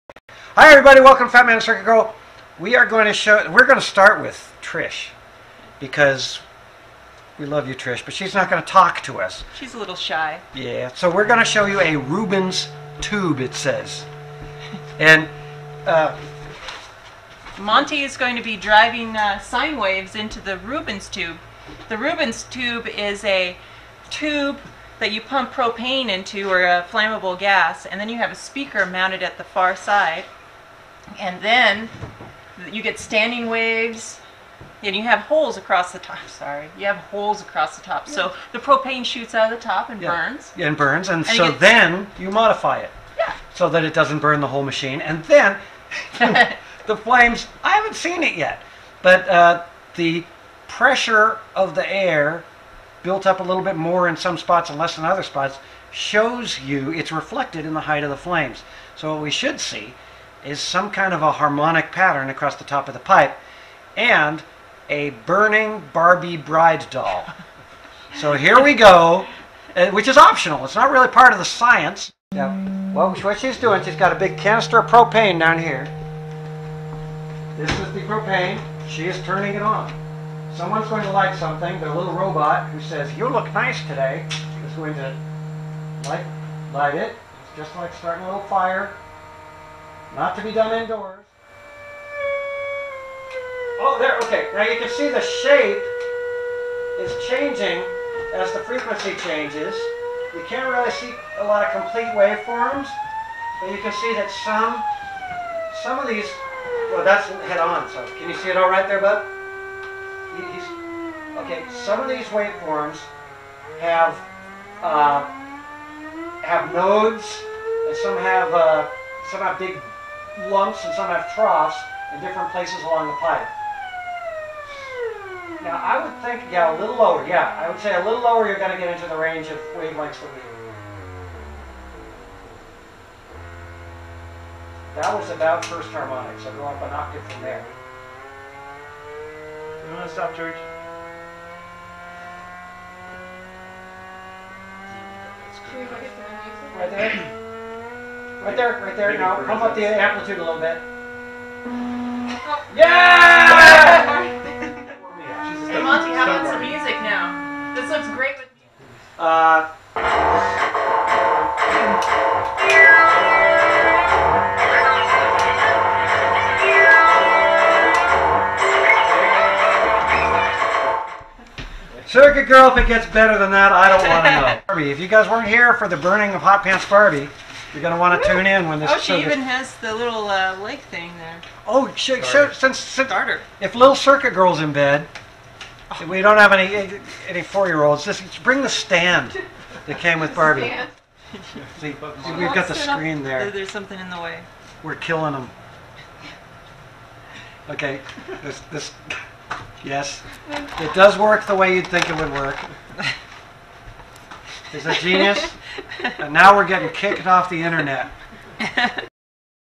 Rubins Tubes - Hot Pants Barbie
Trish builds a Rubin's Tube to demonstrate standing sound waves and burns a "Wedding Day Barbie" in the process.  Barbie will be auctioned for charity along with many of the other FMCG prototypes.